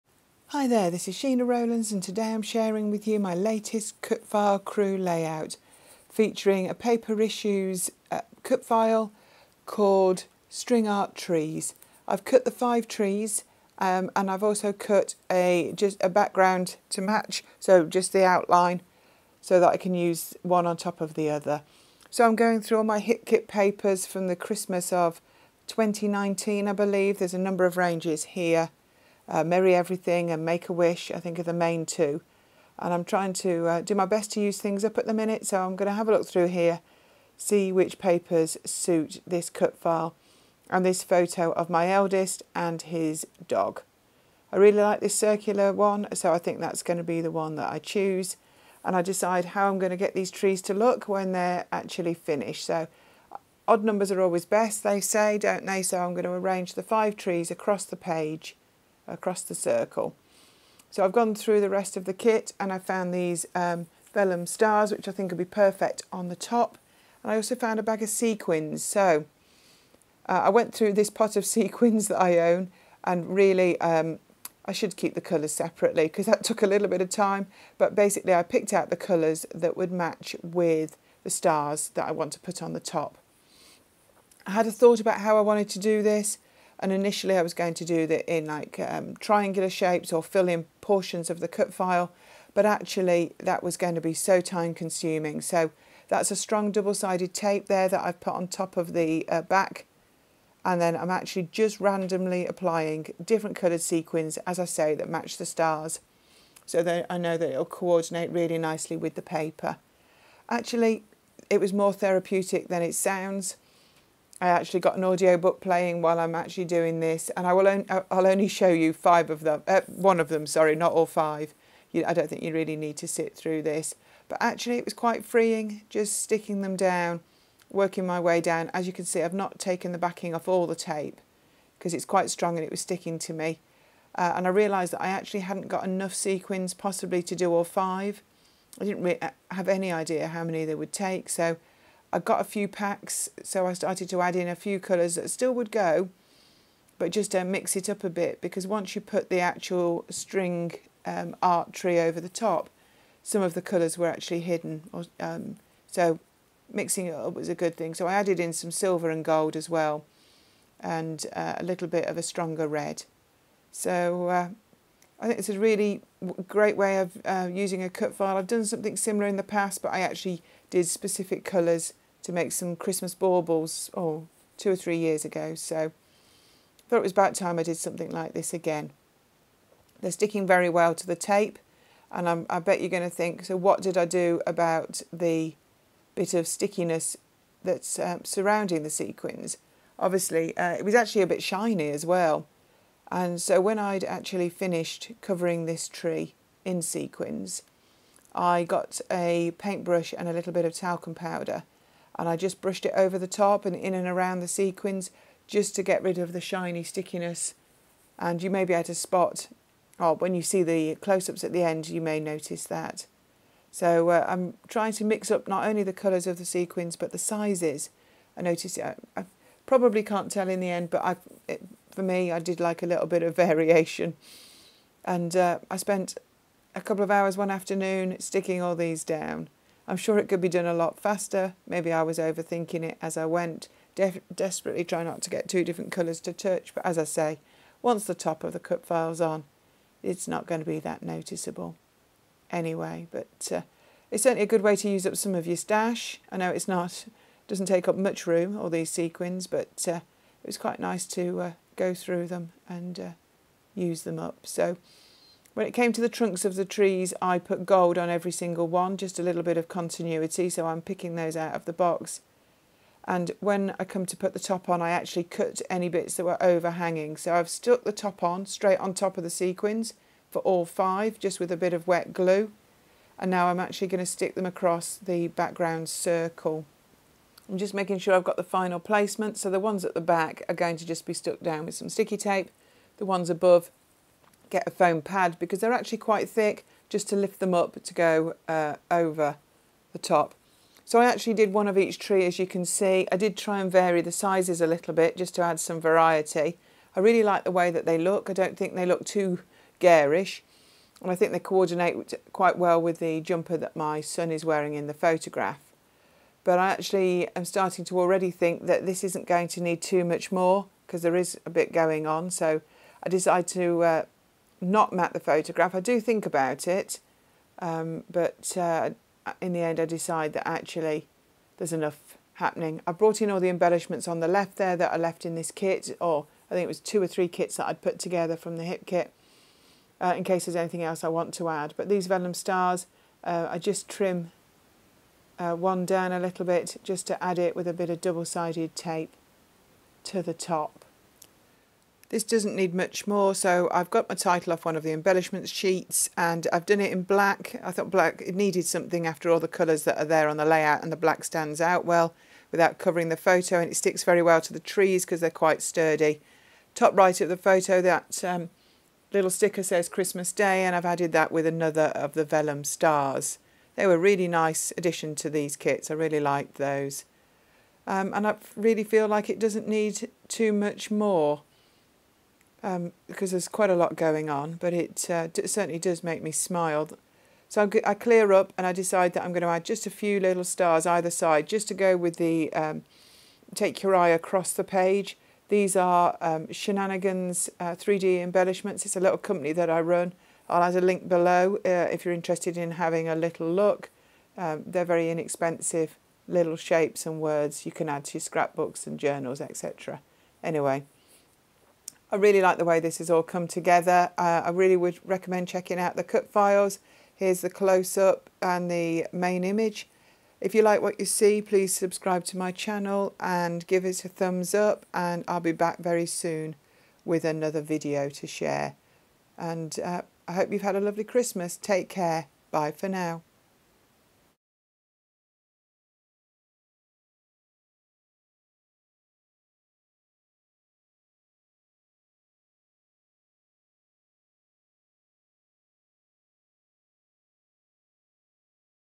Scrapbook process video cutfilecrew monthly challenge featuring paperissues hipkitclub kits
Today I’m sharing my latest video for cutfilecrew a challenge group on IG who use free cut files and challenge themselves monthly to create with them.

This month it is the String Art Trees cut file from Paper Issues which I’ve used with Hip Kit products from Christmas 2019 and 3D stars my little embellishments business.